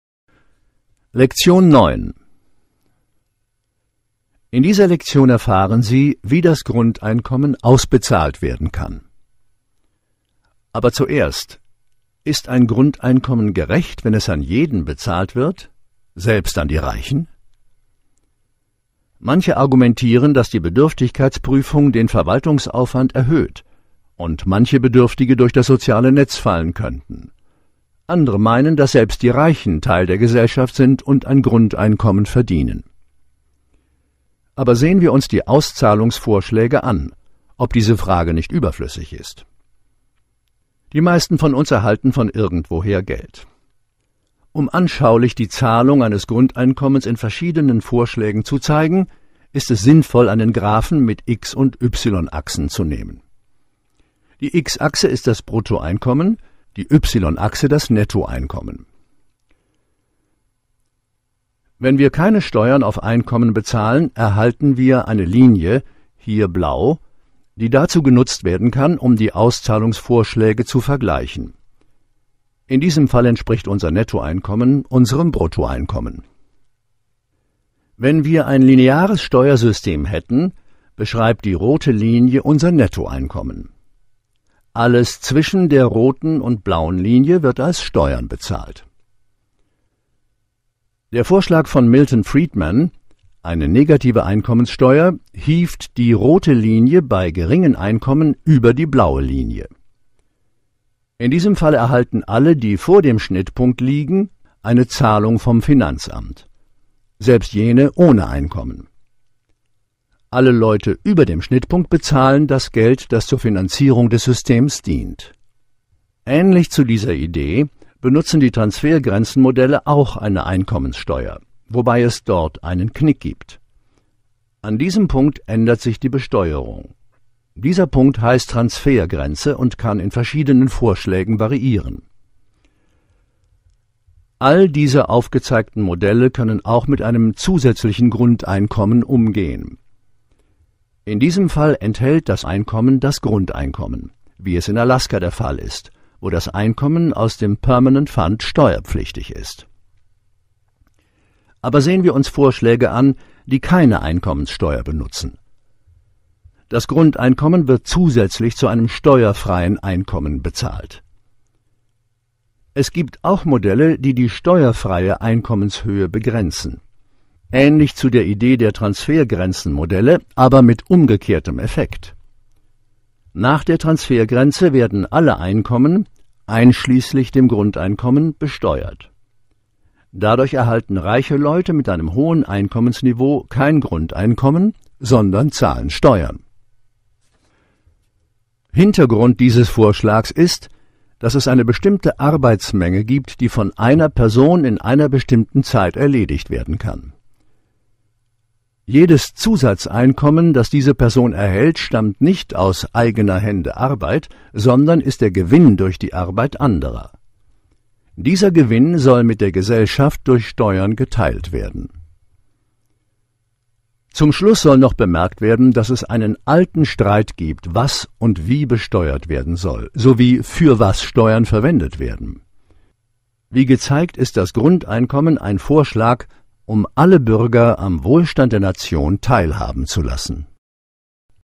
09 | Ausbezahlung
In dieser Lektion erfahren Sie, wie das Grundeinkommen ausbezahlt werden kann
Sprecher: Fritz Stavenhagen